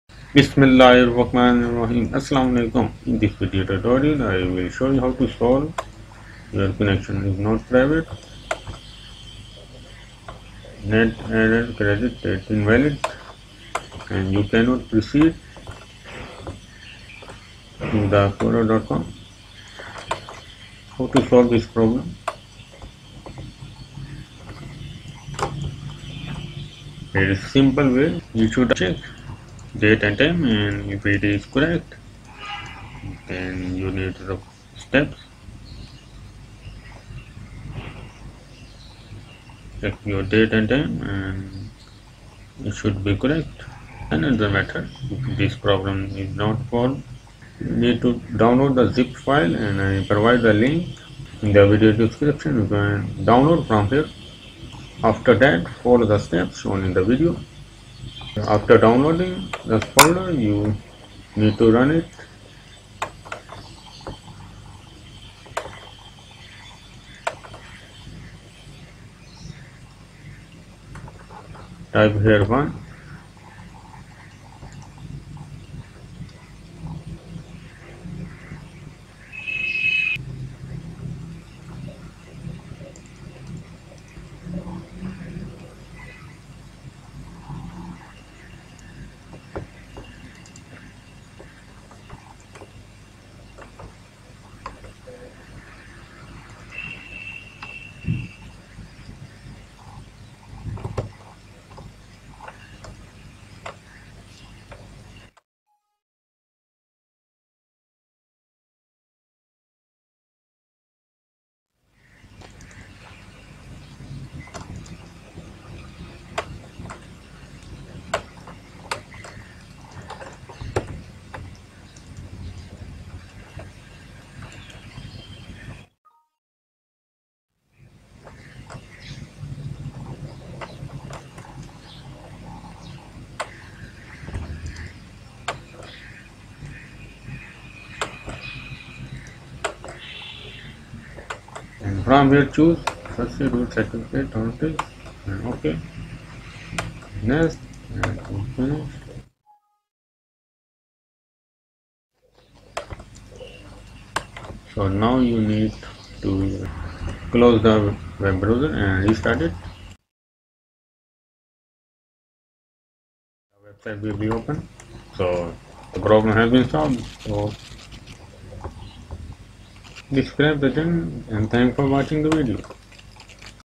How To Fix "Your Connection Is Not Private Google Chrome" Error / Problem Easily
The Video Tutorial Shows You How To Fix / Solve "Your Connection Is Not Private" "net::err_cert_date_invalid Error" - "Privacy Error" - "Attackers might be trying to steal your information And you cannot right now because the website uses hsts. network errors and attacks are usually temporary, so this page will probably work later" In Google Chrome Running Windows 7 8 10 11 Easily.